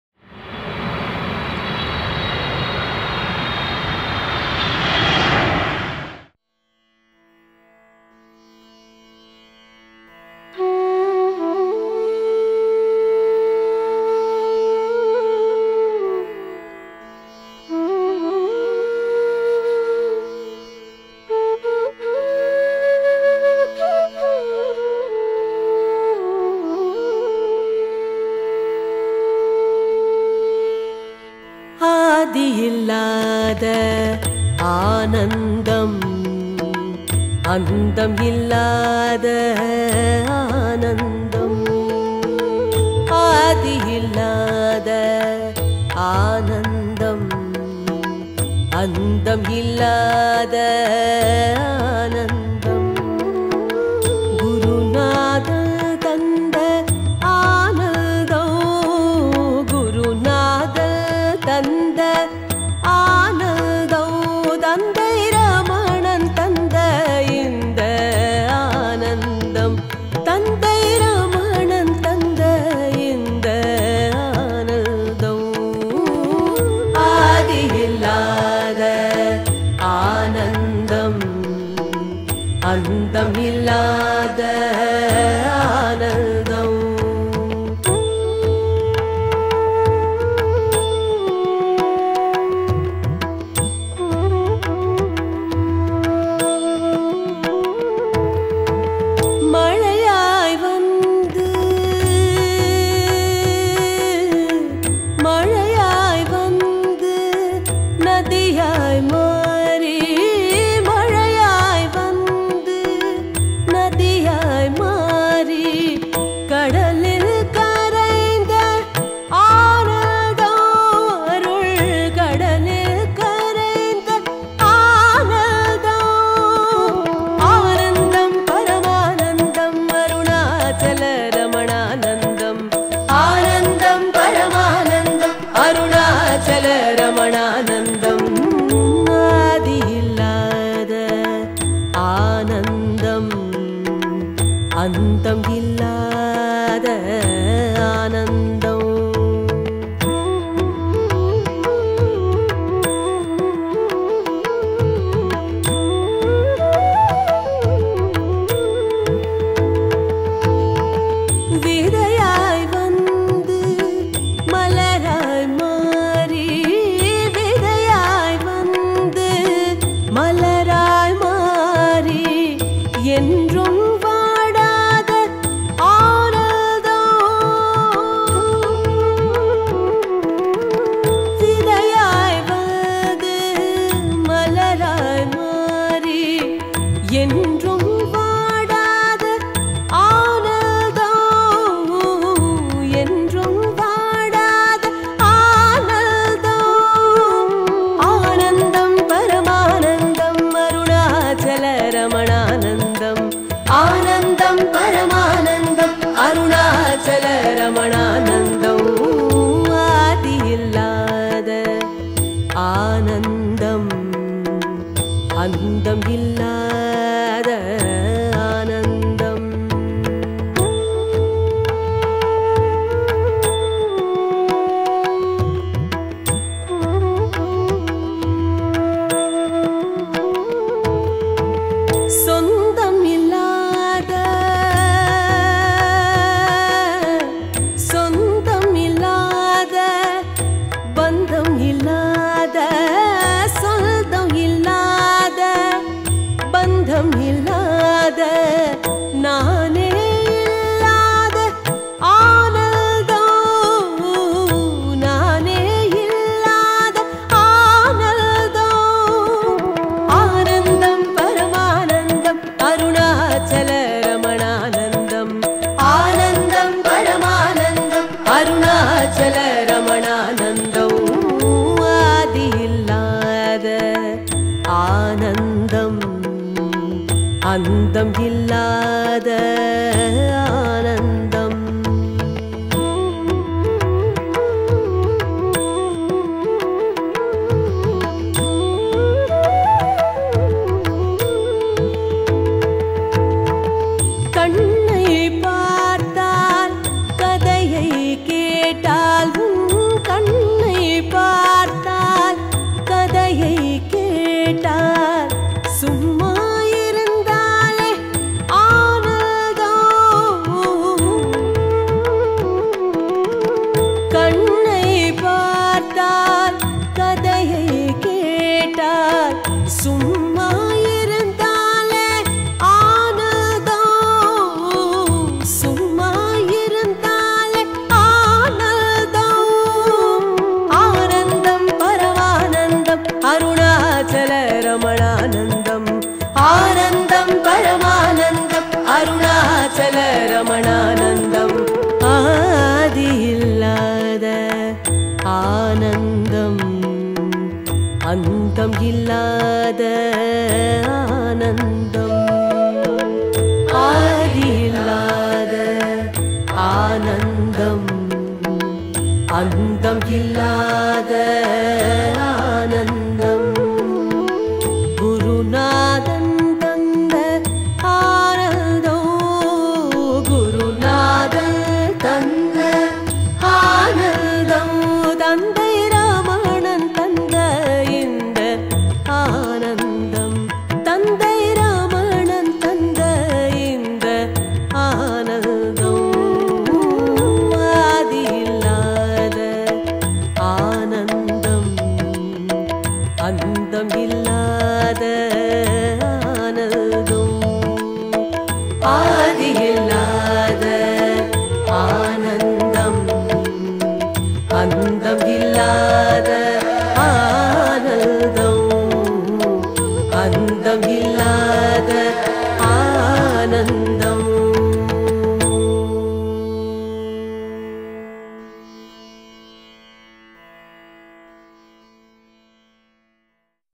Soham Ashram
Life in Soham Ayur Yoga Ashram, Bangalore.
Yoga Teachers Training Course in Ashram.